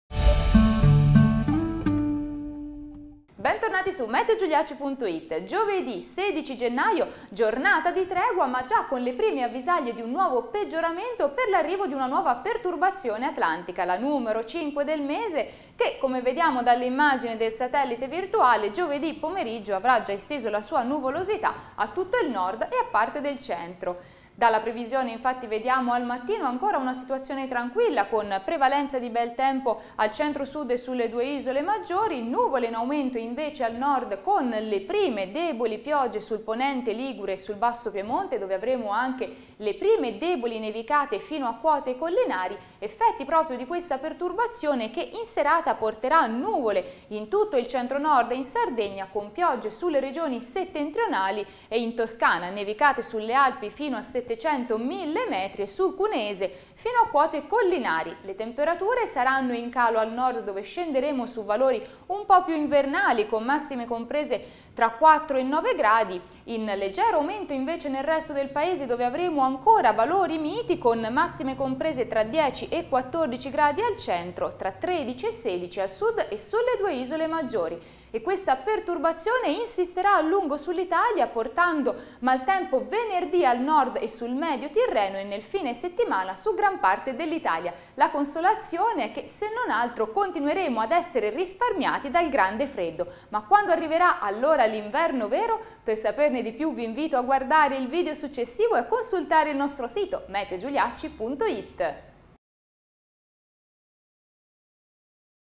Meteo di Giovedì 16 Gennaio.
Le previsioni del Colonnello Mario Giuliacci per Giovedì 16 Gennaio.